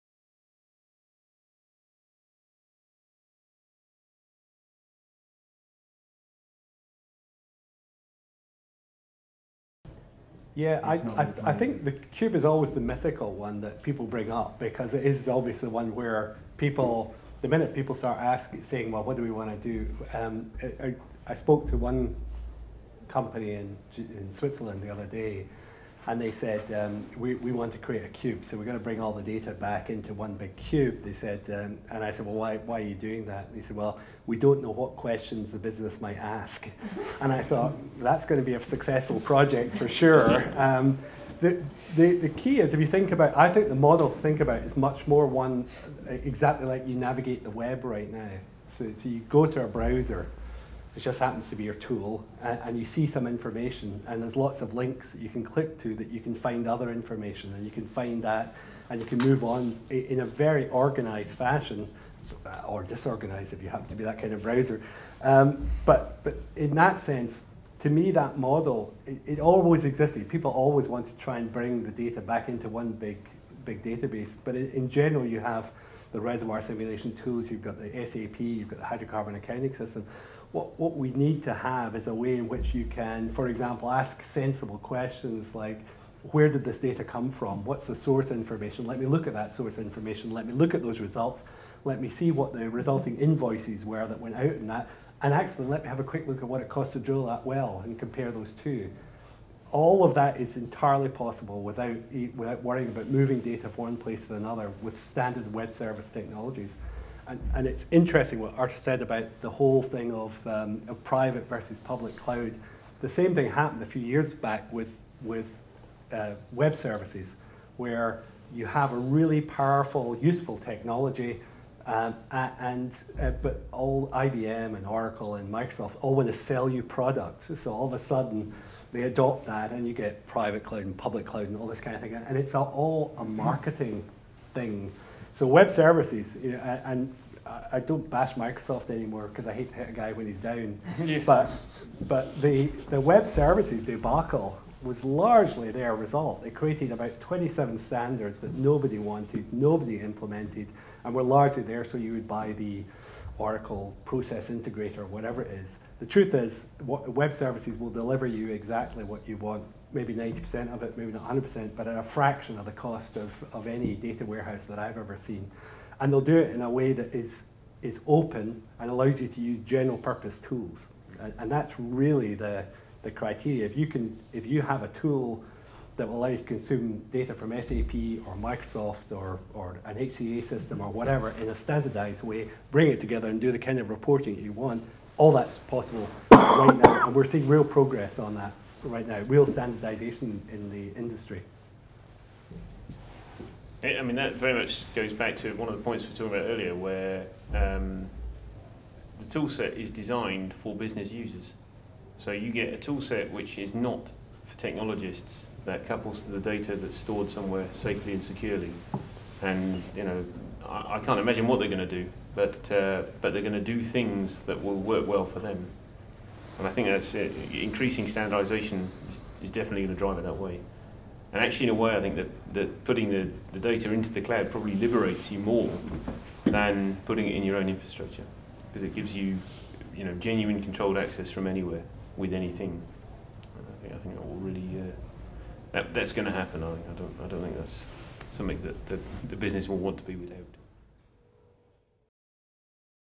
Data Standards in Oil and Gas
The audience and panel from our recent 'Addressing the Challenges in Hydrocarbon Allocation' share opinions around data standards in oil and gas.